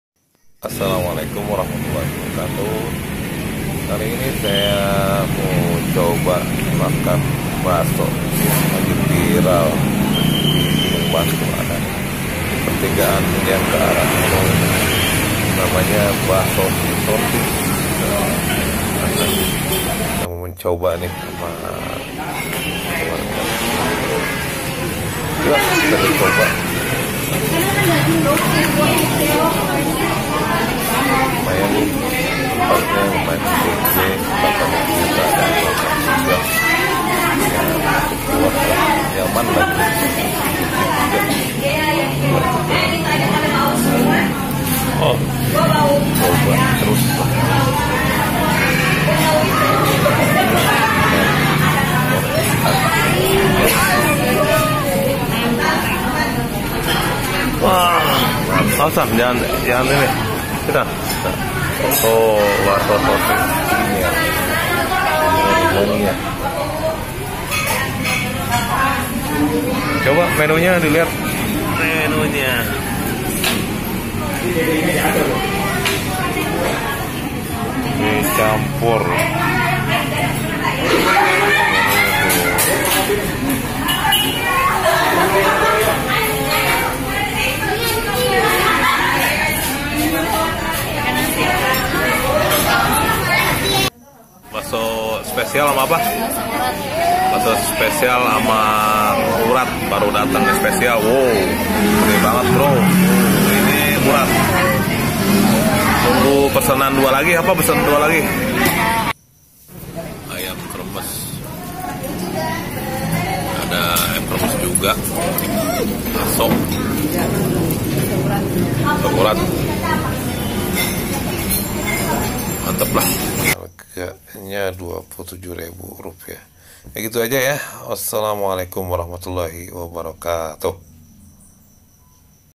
Baso Titoti di jalan Loji Bogor
Baso Titoti yang viral ini terletak di jalan loji dekat pertigaan lampu merah di jalan Darul qur'an Bogor, tidak jauh dari kantor kecamatan Bogor barat.

Lumayan rame pengunjungnya karena bersaing juga dengan Baso Gunung kidul yang tak kalah viral.